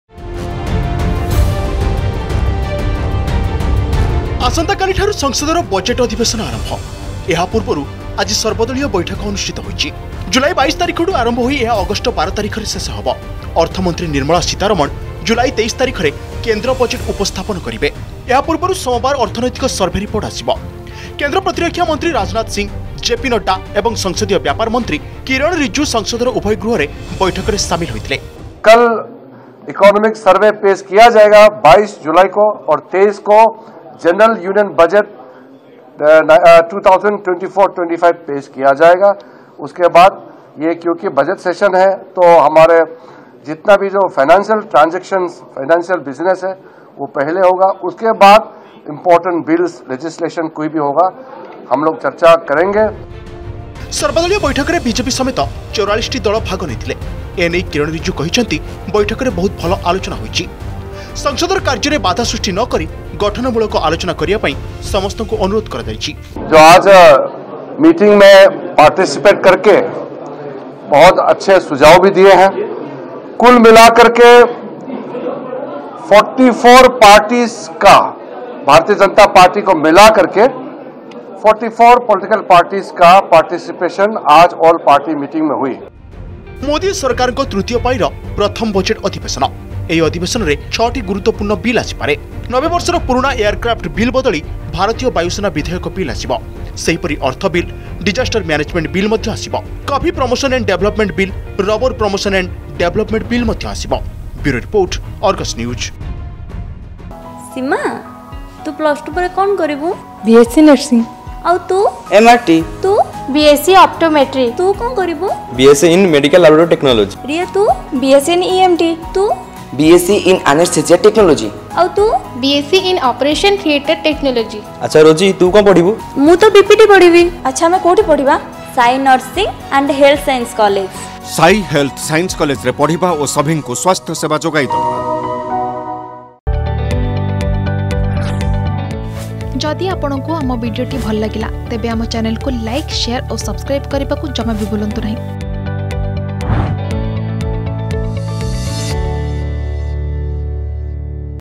ସୋମବାର ଠାରୁ ସଂସଦର ବଜେଟ ଅଧିବେଶ ଆରମ୍ଭ ହେବ || All Party Meeting
ସୋମବାର ଠାରୁ ସଂସଦର ବଜେଟ ଅଧିବେଶ ଆରମ୍ଭ ହେବ। ଏହା ପୂର୍ବରୁ ରବିବାର ସର୍ବଦଳୀୟ ବୈଠକ ଅନୁଷ୍ଠିତ ହୋଇଛି। ଜୁଲାଇ 22 ତାରିଖରୁ ଆରମ୍ଭ ହୋଇ ଏହା ଅଗଷ୍ଟ 12 ତାରିଖର ଶେଷ ହେବ। ଅର୍ଥ ମନ୍ତ୍ରୀ ନିର୍ମଳା ସୀତାରମଣ ଜୁଲାଇ 23 ତାରିଖରେ କେନ୍ଦ୍ର ବଜେଟ ଉପସ୍ଥାପନ କରିବେ।